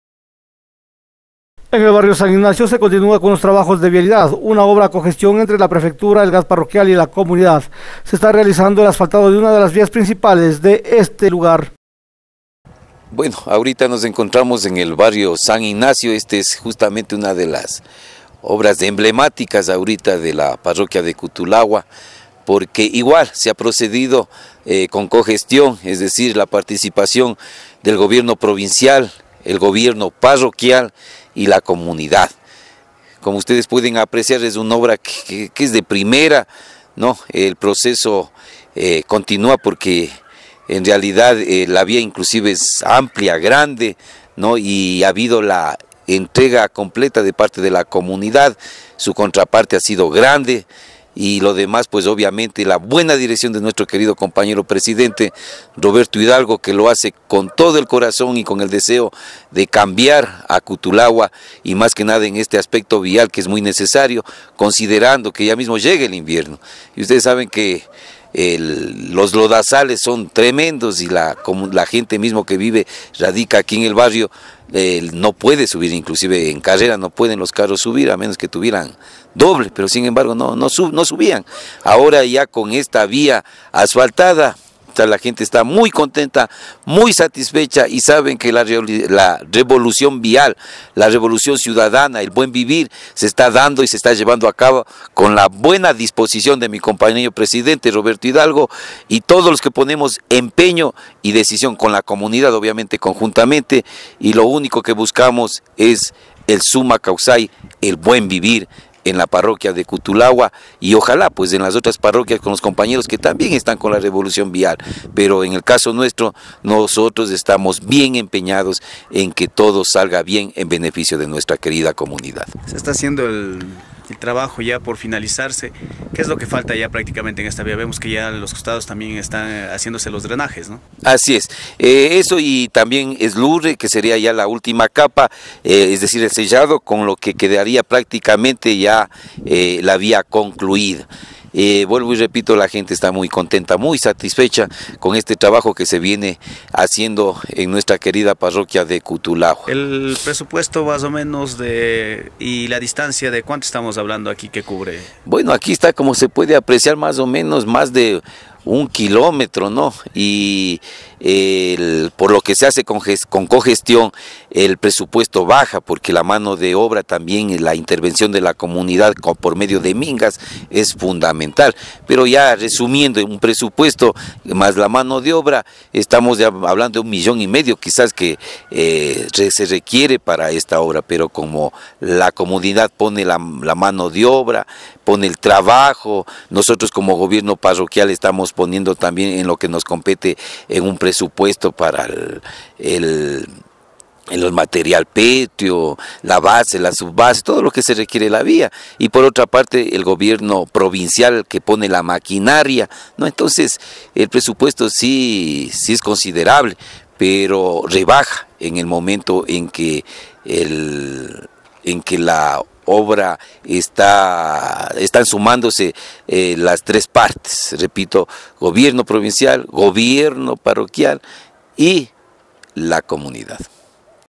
OBRA EMBLEMÁTICA EN EL BARRIO SAN IGNACIO
EN EL BARRIO SAN IGNACIO SE CONTINUA CON LOS TRABAJOS DE VIALIDAD, UNA OBRA A COGESTION ENTRE LA PREFECTURA EL GAD PARROQUIAL Y LA COMUNIDAD, SE ESTA REALIANDO EL ASFALTADO DE UNA DE LAS VÍAS PRINCIPALES DE ESTE SECTOR.